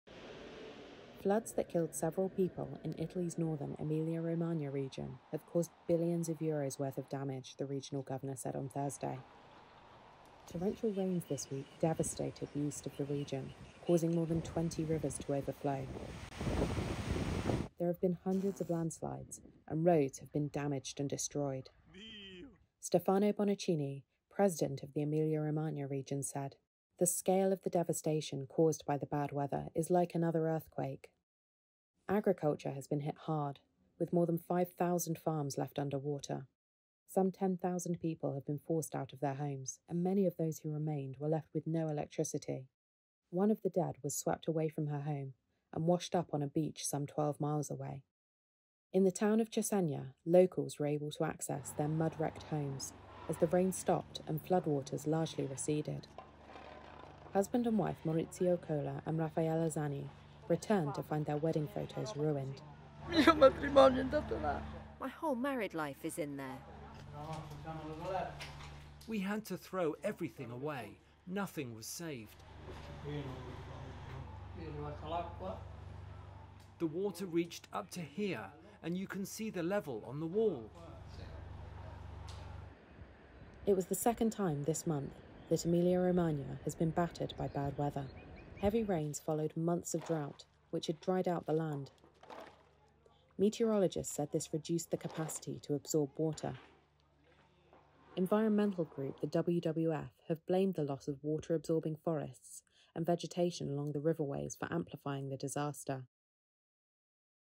「地震規模の被害」 イタリア北部で豪雨により洪水や地滑り、9人死亡
イタリア北部エミリアロマーニャ州を襲った豪雨により洪水や地滑りが発生し、９人が死亡した。洪水被害は３７市町村に及び、数千人が避難を強いられる事態となっている。ボナッチーニ州知事は「損害の規模はまるで地震のようだ」と述べた。世界自然保護基金（ＷＷＦ）は、水を吸収する森林や川沿いの植物失われたことが、災害を拡大させたとの見方を示した。